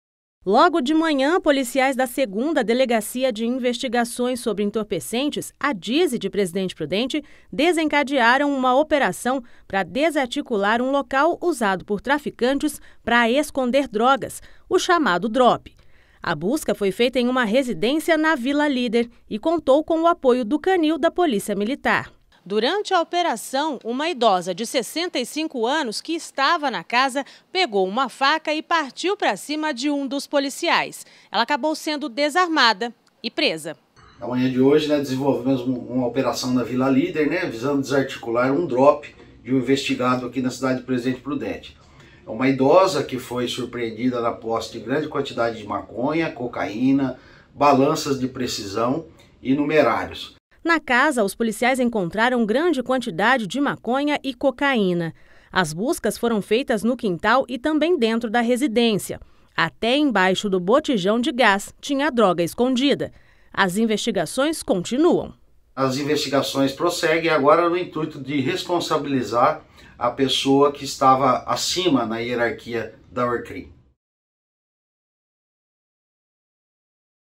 Idosa tenta agredir policial com faca em Presidente Prudente
Uma ação da Polícia Civil, em Presidente Prudente, realizada para desarticular um esconderijo de drogas, terminou com a prisão de uma idosa. Ela teria tentado agredir o policial com uma faca.